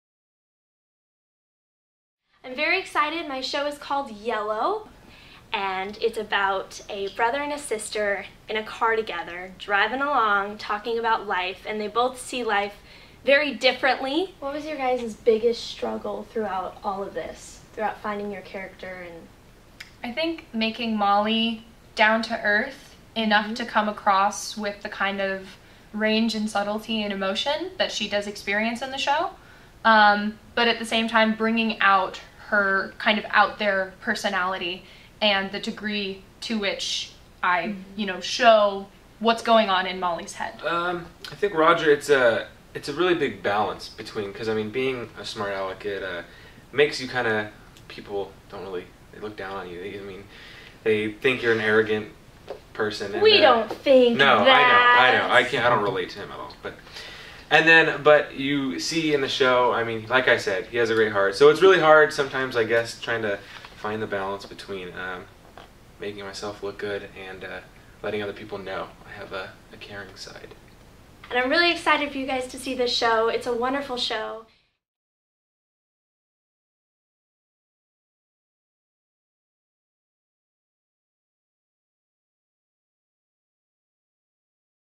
Acting Challenges in Show Off's mini-play Yellow
Director Sabrina Paris talks with cast members Emma Hutchinson and Ian Rattray about the acting challenges they faced in the upcoming play Yellow. Yellow is one of seven short plays to be presented when Show Off opens next Friday, Jan. 15.